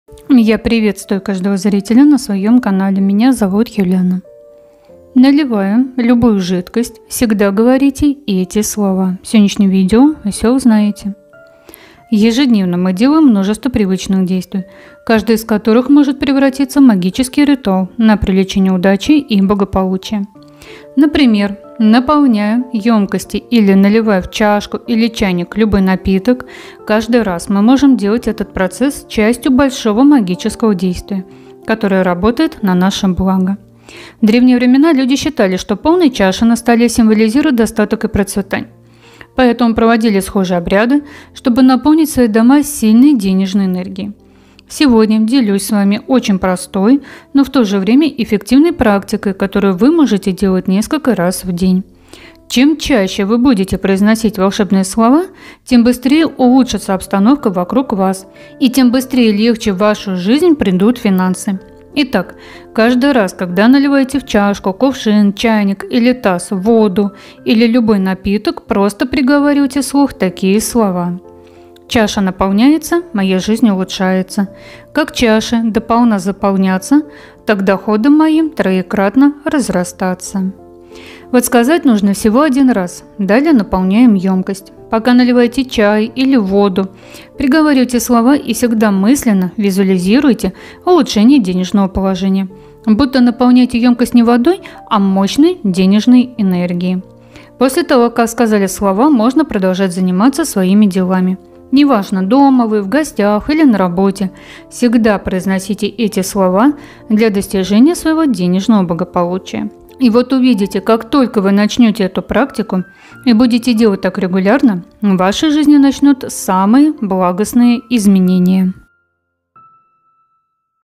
Всегда говорите эти слова на воду - На благо.
Ежедневно мы делаем множество привычных действий,
каждое из которых может превратиться в магический ритуал
на привлечение удачи и благополучия.

Слова:
«Чаша наполняется — моя жизнь улучшается.
Как чаше до полна заполняться, так доходам моим троекратно
разрастаться!»

Благодарю Вас!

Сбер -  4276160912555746